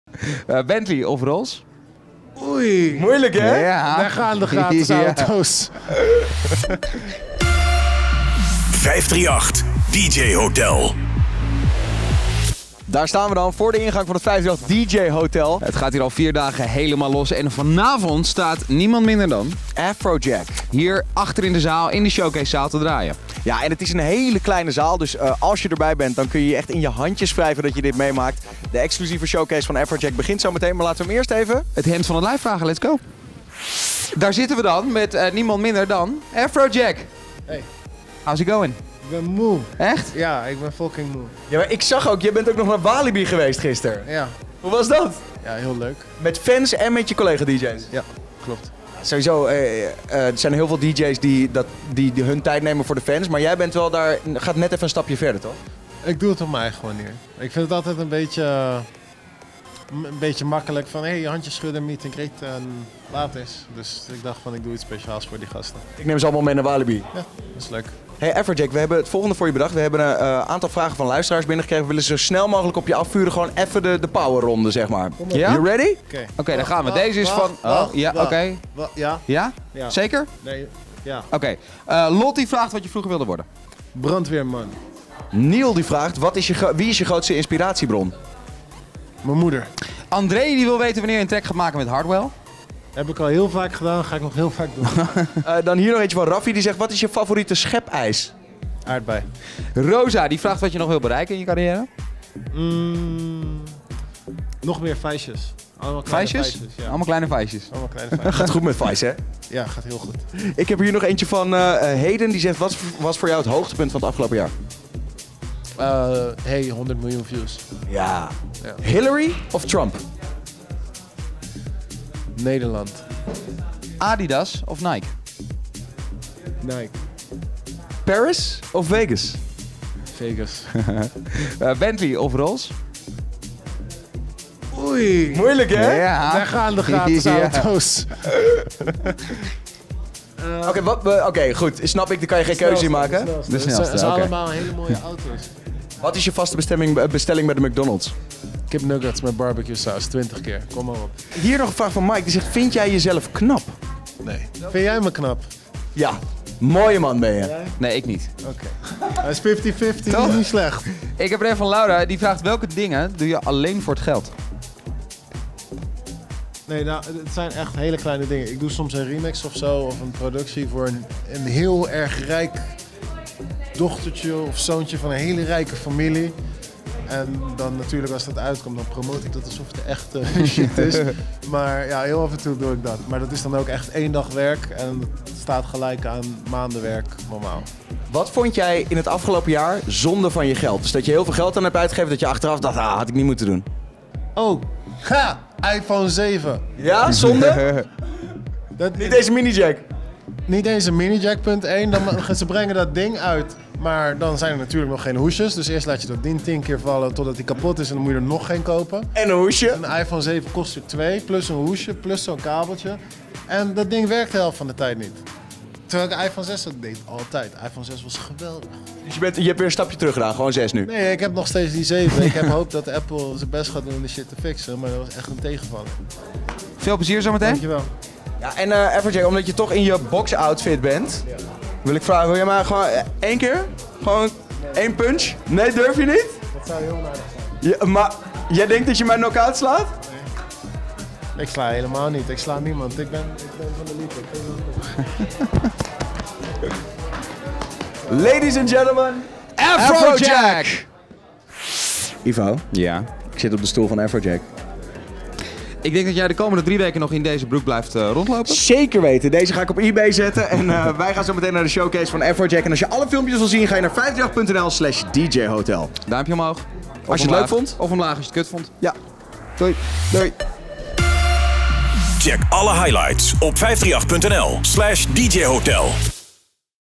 Ask Afrojack Anything | 538DJ Hotel 2016
In Ask Afrojack Anything geeft de dj overal antwoord op. Wat is z’n favoriete schepijs? Bentley of Rolls Royce? Wat is z’n standaard bestelling bij de McDonalds? En welke dingen doet-ie alleen voor het geld? Check de video en je weet het.

Meer zien van het 538DJ Hotel? Check